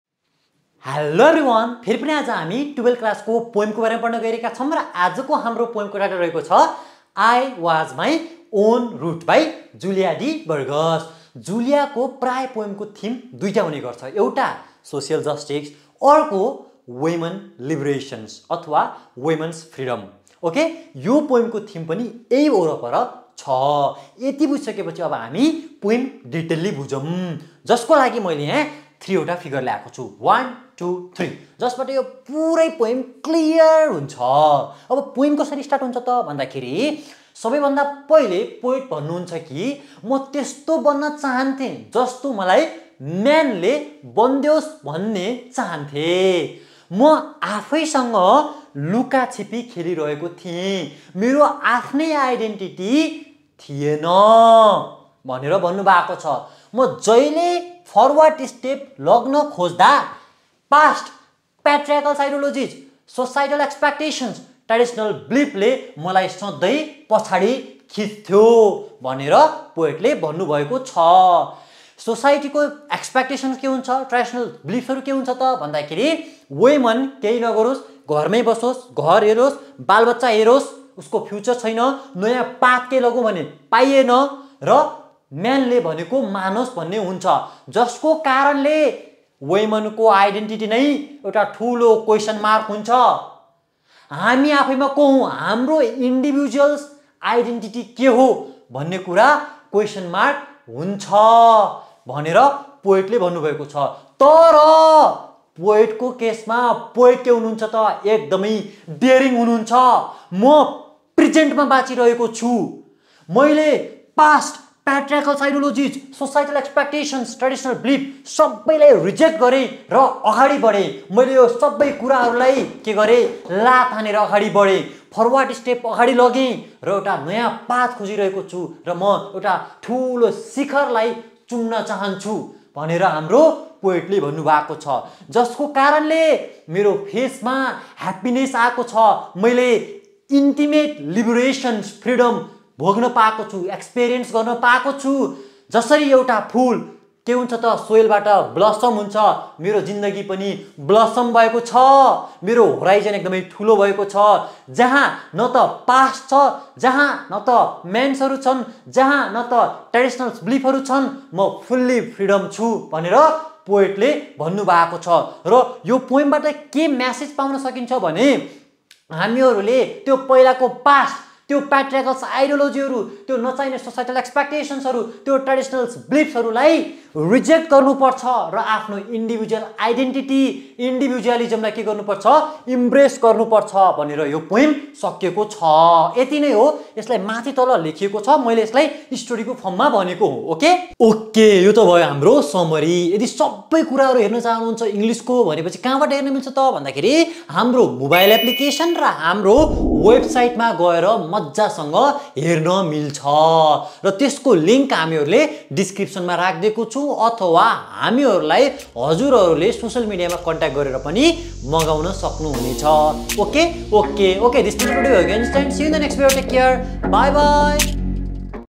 I Was My Own Route | By Julia de Burgos | Class 12 Compulsory English | NEB | ELO Peeth
The poem "I Was My Own Route" by Julia de Burgos explores the struggle of women to break free from societal expectations and patriarchal norms that confine them to predetermined roles. The poet describes her desire to conform to the expectations imposed by men but realises that she is made of the present moment, refusing to walk backwards or conform to outdated ideologies. Despite facing resistance and pressure from the old guard, representing traditional beliefs, she embraces her forward-moving journey. With each step, she distances herself from past constraints, allowing her identity to expand and liberate itself from societal constraints. Ultimately, she finds empowerment in defining her own path and embracing the diversity of experiences and influences that shape her existence.